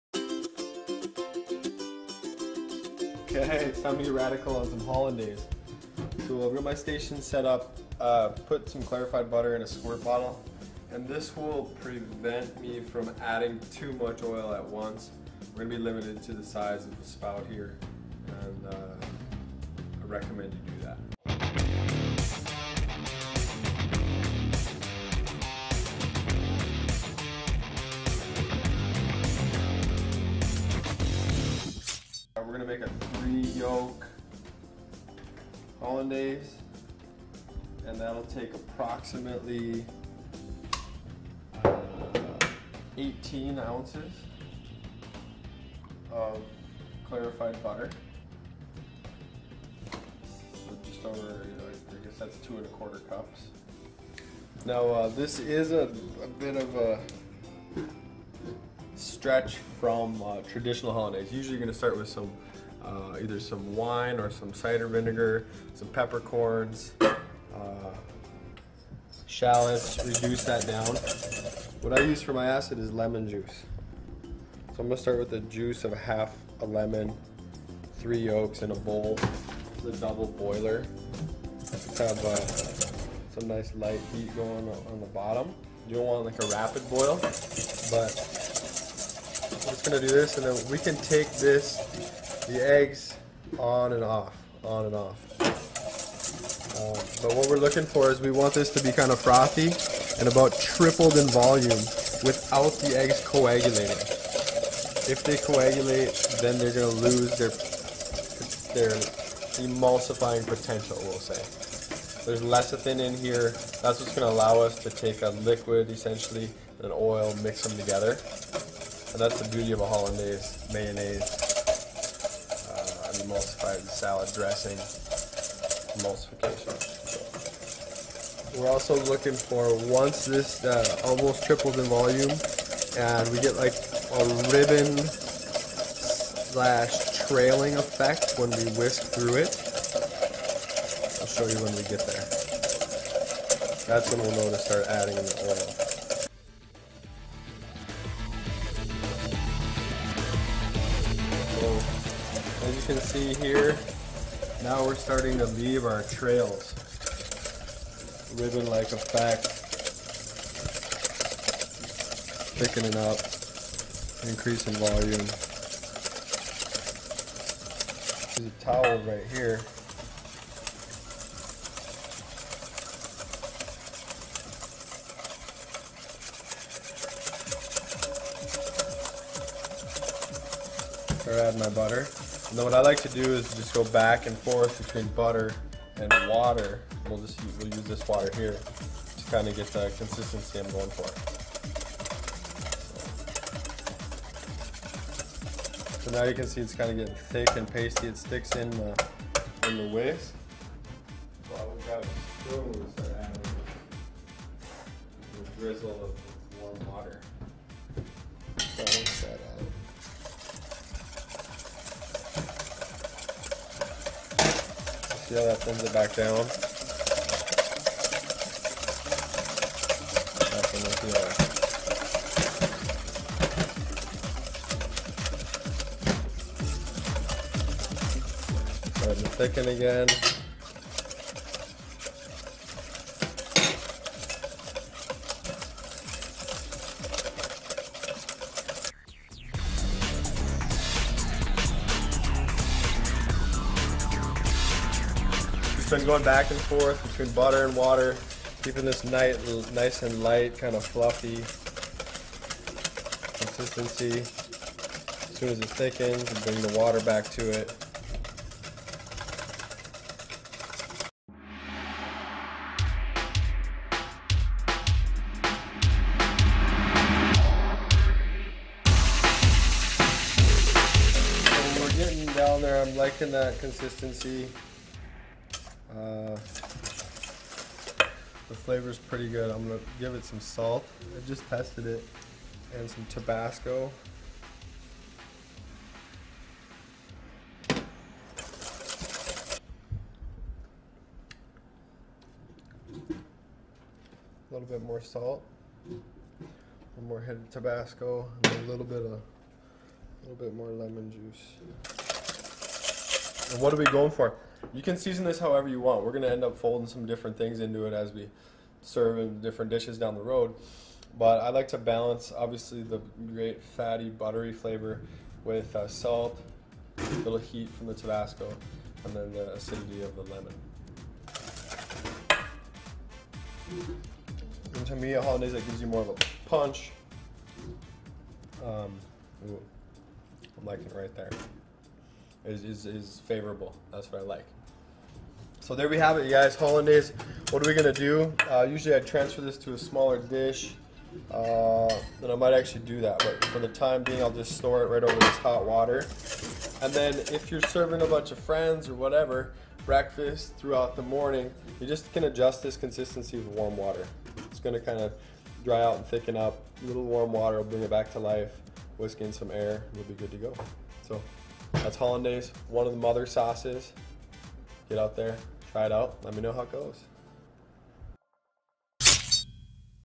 Holly Holly Hollandaise - Paleo Cooking with Nick Massie
Now that you know how to make clarified butter, your pump is primed for hollandaise! One of the mother sauces, hollandaise deserves a spot on your bucket list. Any cook or culinary school graduate worth their weight should be able to pump out a tasty, velvety hollandaise in no time.